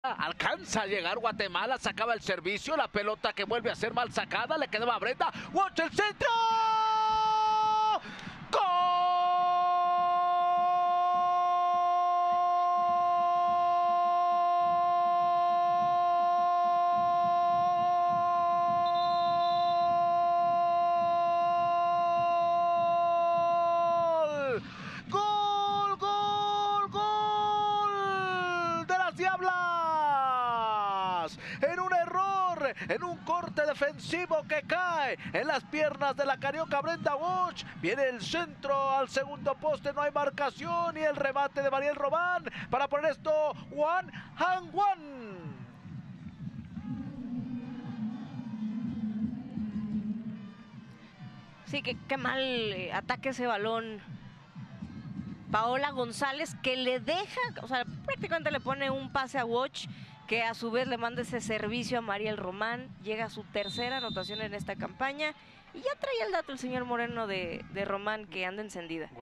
Gol de Mariel Román - J14 | Clausura 2023
¡¡¡G⚽⚽⚽L!!!

👹 Brenda Woch recuperó el balón, y con una gran asistencia, Mariel Román remató de cabeza para poner el empate.